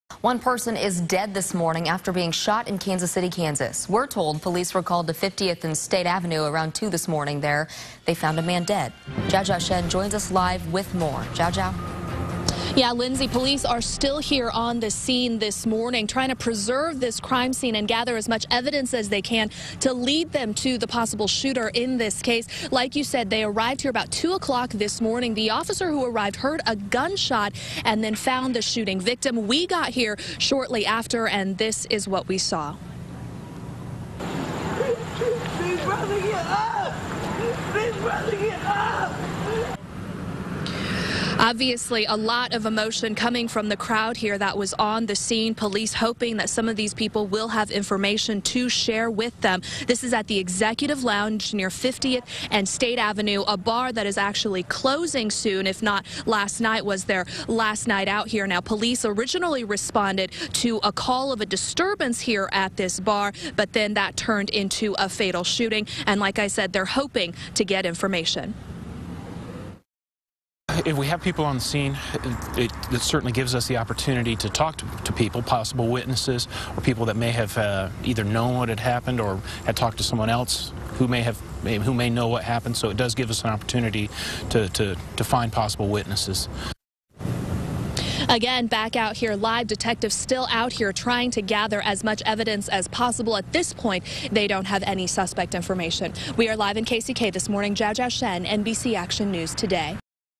Cops called to disturbance heard gunshots
KCK police officers called to a disturbance early Thursday heard gunshots and found a man dead.